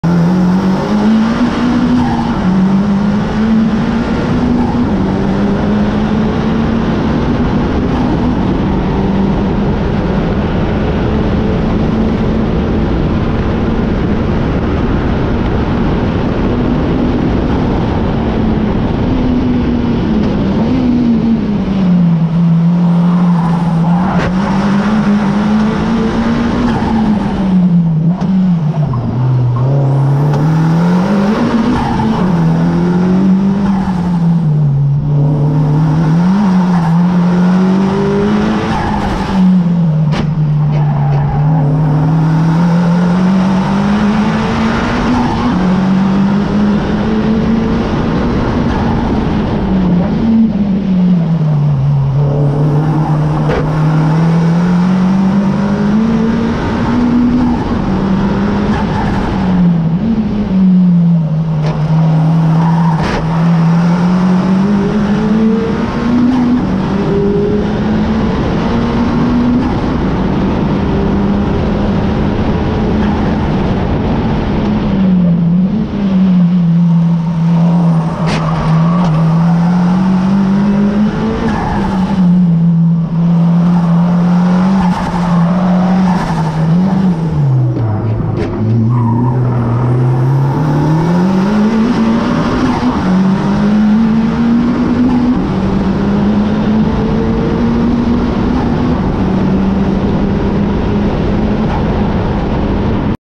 Acura Grand Prix of Long Beach | Civic Type R FK8
A lap around the Acura Grand Prix of Long Beach with Global Time Attack.

Car: 2017 Honda Civic Type R